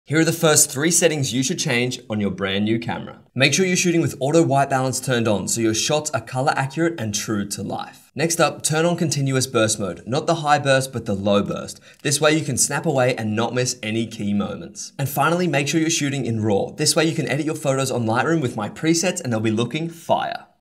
3 Things YOU Should Change on a New Camera
3 Things YOU Should Change on a New Camera

These photography videos are usually directed at beginner photographers. Take the photography tips and tricks with a grain of salt and try to understand the tips from a beginner photographer's point of view.